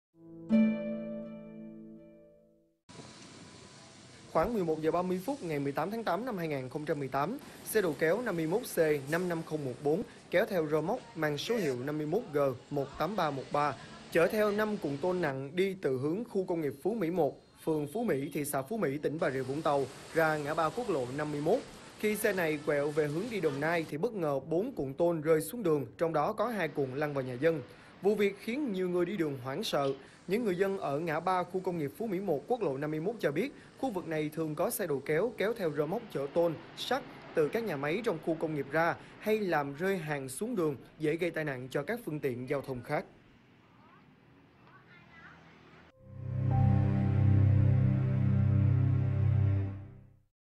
Hú vía vì 4 cuộn tôn “khủng” rơi xuống đường, lăn vào nhà dân
Xe đầu kéo kéo theo rơ moóc chở 5 cuộn tôn nặng hàng chục tấn đang đi trên đường bất ngờ rơi xuống đường, lăn vào nhà dân.